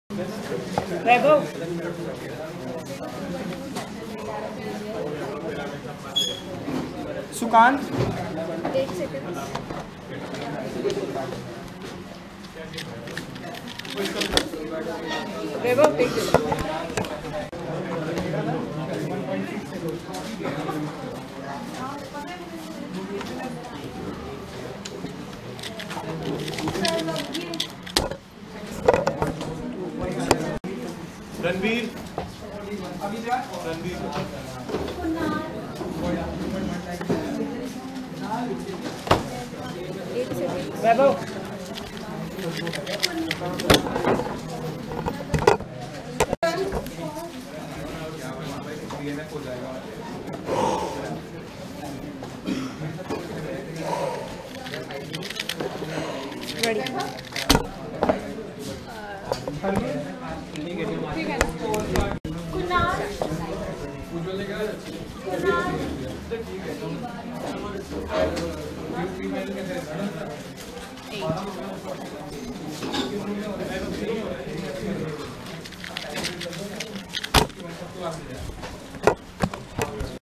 Official 2x2 Average: 2.46!
1.88, 2.79, 2.36, 3.33, 2.24 = 2.46 avg :)
My 7th best official avg :p not bad considering I haven't practiced 2x2 since 2017 :p

cube- Valk2
SRCC 2019